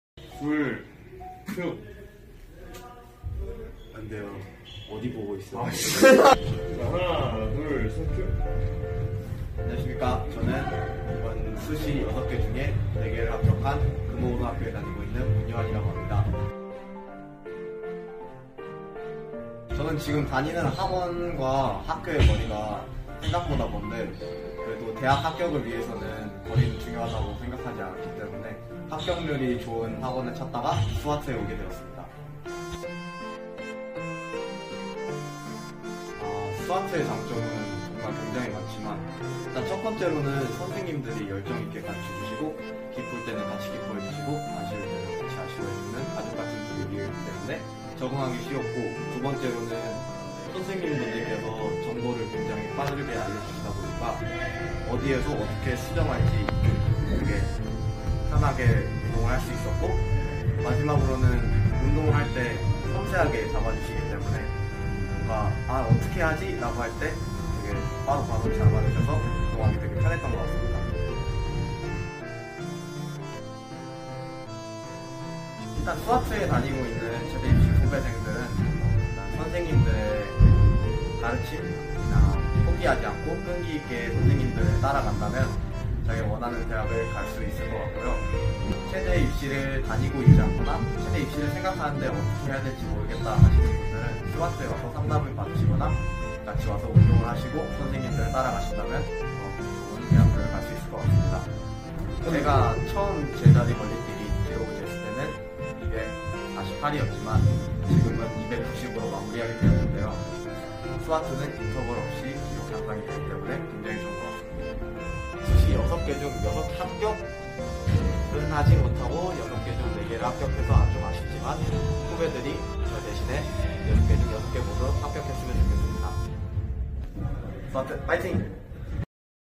4개월만 준비해서 수시로 체대합격!!ㅣ금호고등학교 문요한 [스왓TV]
4개월 준비해서 수시로 대학 합격한 금호고 3학년 문요한!!
그의 합격 이야기를 들어보자!!

lovesanglee0